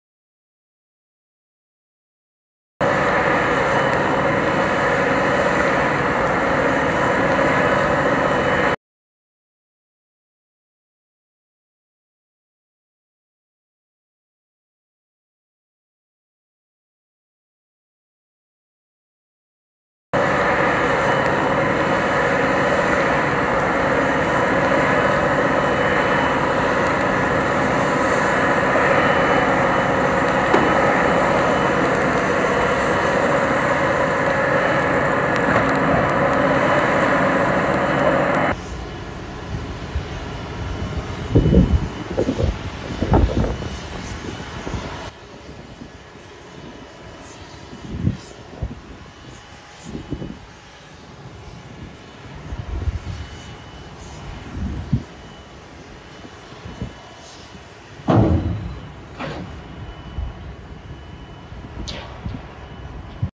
unified command continues to clear steel debris on the M/V Dali
Salvors with the Unified Command continue wreckage removal from the M/V DALI, May 4, 2024, during the Key Bridge Response 2024.

Debris and wreckage removal is ongoing in support of a top priority to safely and efficiently open the Fort McHenry Channel.